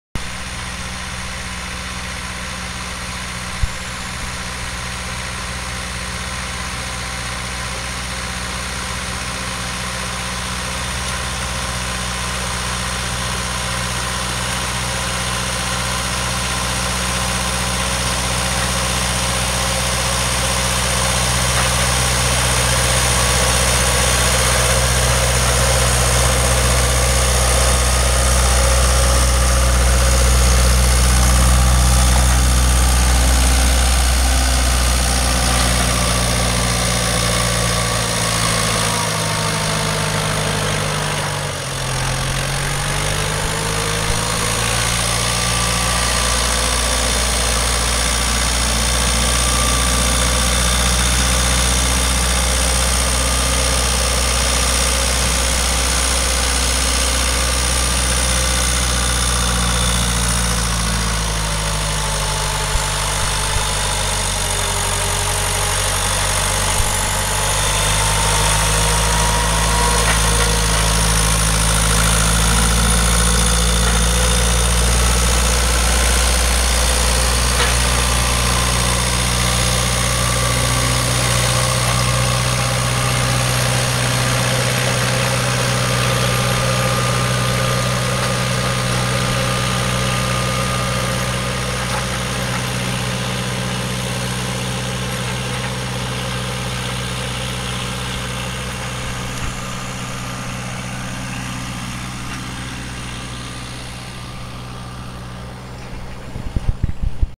Weed Badger 01
Our Weed Badger(tm) at work in a young organic orchard.  This is probably the most valuable weed management tool in our arsenal - especially with young trees.